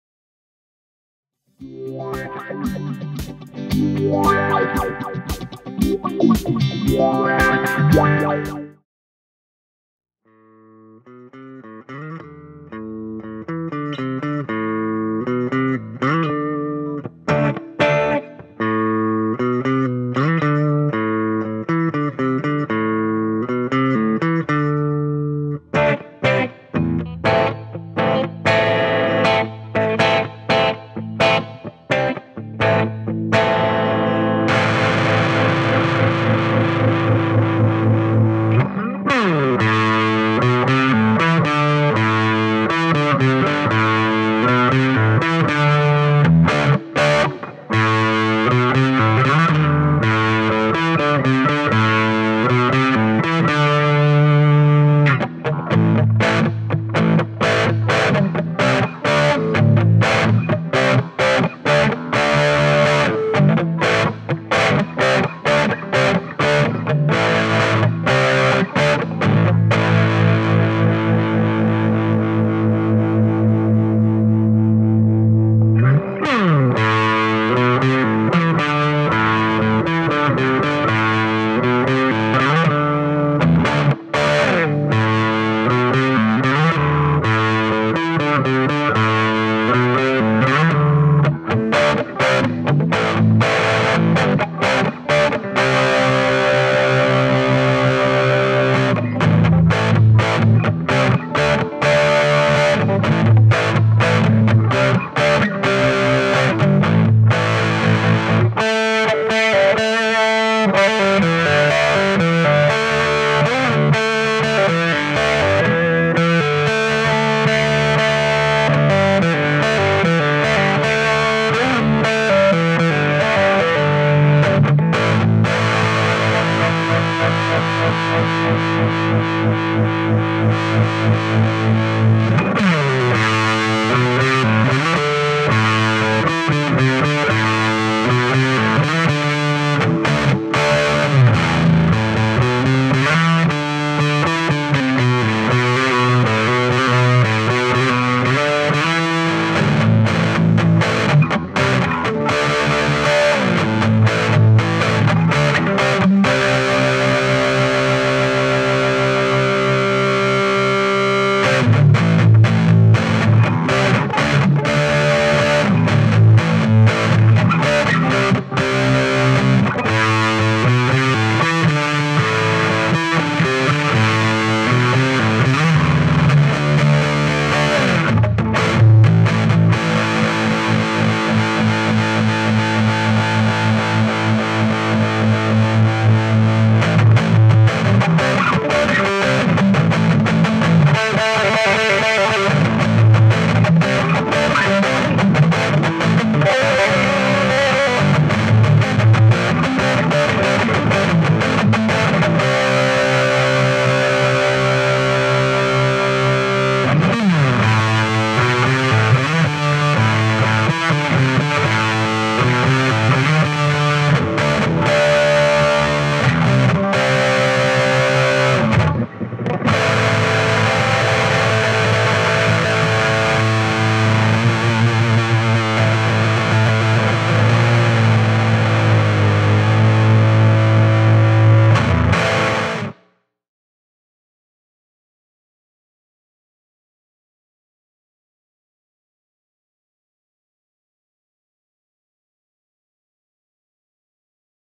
ADAG AUDIO Metal Shock Modern High Gain Distortion
This is a demo video of the Metal Shock Modern High Gain Distortion effects pedal from ADAG AUDIO performed and produced by Pedal of the Day. for more effects pedal reviews and demos, and be sure to check out more of the videos on our YouTube channel as well. Cheers!

Recorded (in stereo) with:

Benson Amps Monarch + 1979 Fender Champ
Speakers by Tone Tubby and Ted Weber